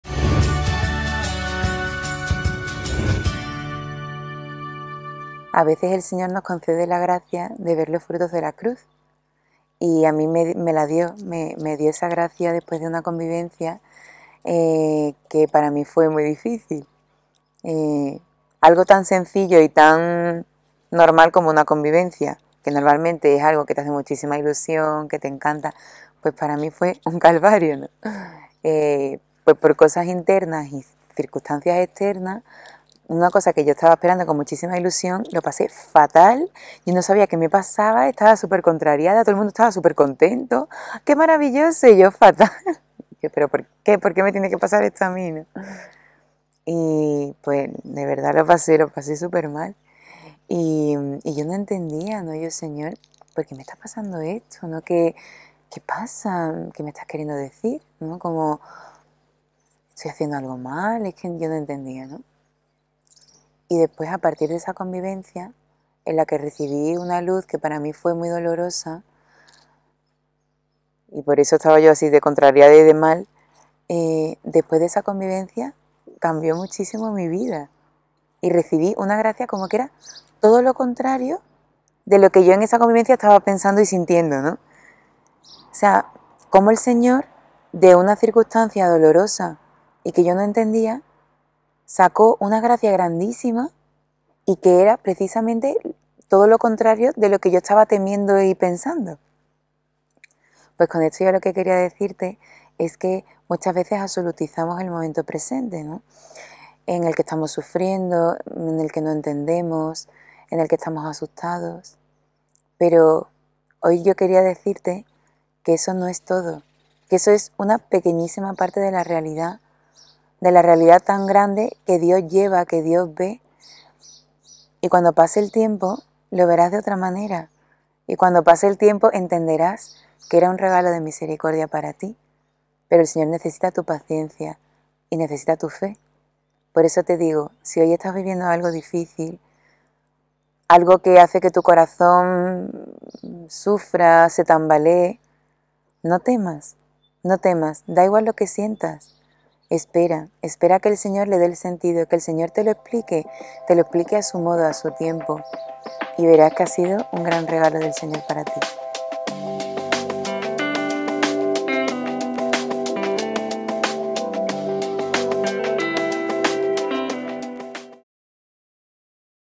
EL SEÑOR NECESITA TU FE | Diosidencias | 𝓗𝓐𝓜
Hola, Paz nos cuenta como el Señor de una circunstancia dolorosa sacó una gracia grandísima...

Frases destacadas de Paz:
"Los frutos de la cruz..." "A veces absolutizamos lo que estamos viviendo pero solo es una pequeña parte..."